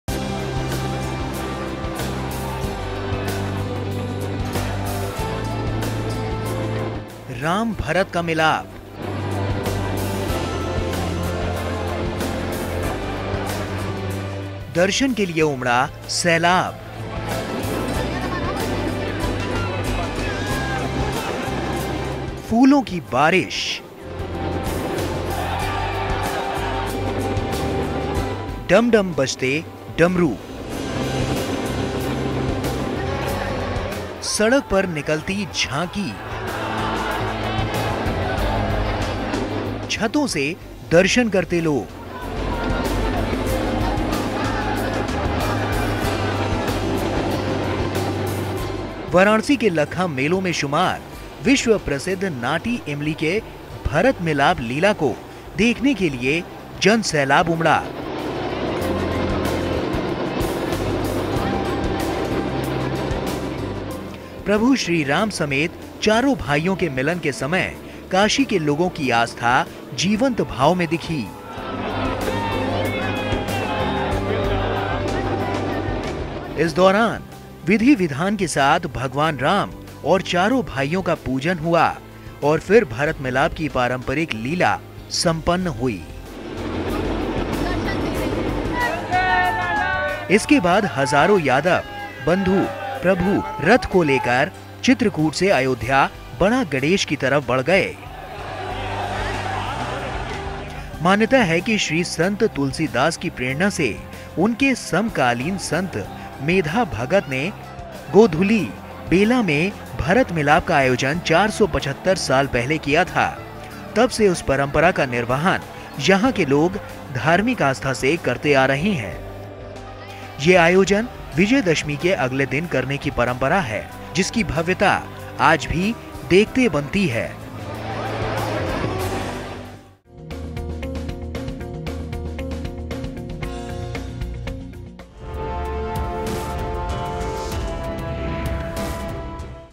475 साल से चला आ रहा है यह ‘भरत मिलाप’!
वाराणसी में भव्य भरत मिलाप का आयोजन हुआ.. जिसके दर्शन के लिए हजारों की संख्या में श्रद्धालु पहुंचे.. मान्यता है कि श्री संत तुलसी दास की प्रेरणा से उनके समकालीन संत मेधा भगत ने गोधूलि बेला में भरत मिलाप का आयोजन 475 साल पहले किया था..